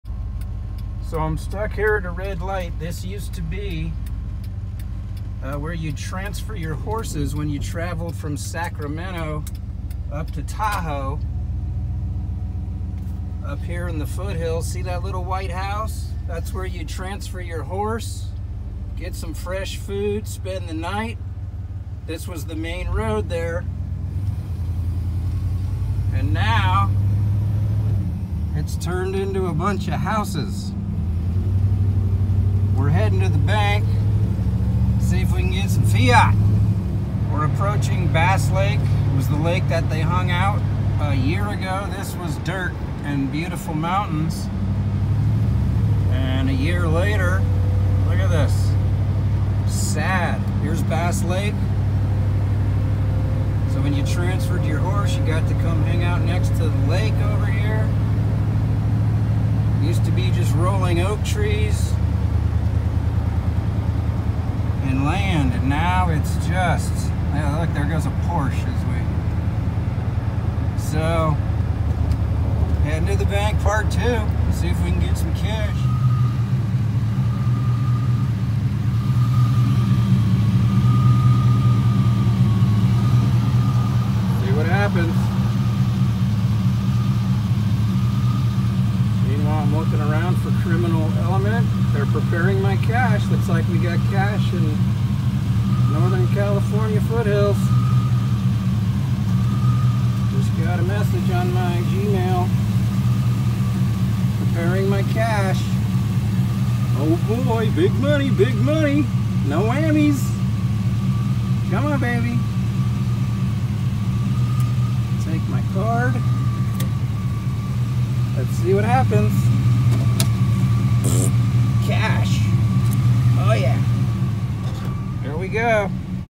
There’s Kash In Them Hills! @SilverandCigars
Well looks like there is still some cash up here in these hills! Also a little History on the horse transfer house, when traveling from Sacramento to the Sierras, this is where you could get a fresh horse, hot meal and place to stay…